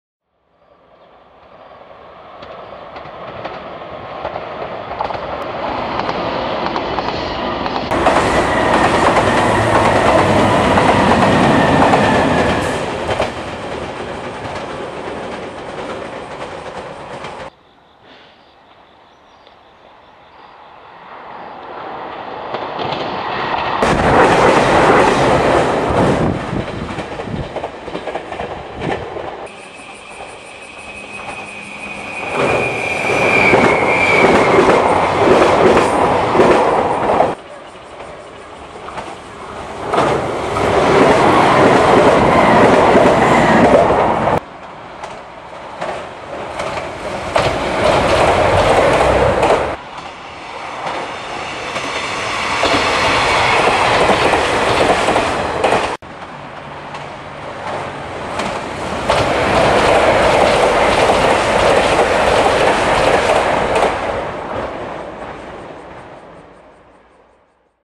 JR四国 市坪駅 列車通過 2020年８月
2020年3月、松山運転所、貨物駅は北伊予駅ー伊予横田駅間に移転。そこに新しい駅「南伊予駅」も運用開始されました。松山駅からそこに向かって一つ目の駅「市坪駅」で通過列車を少し観察。回送列車も見れました。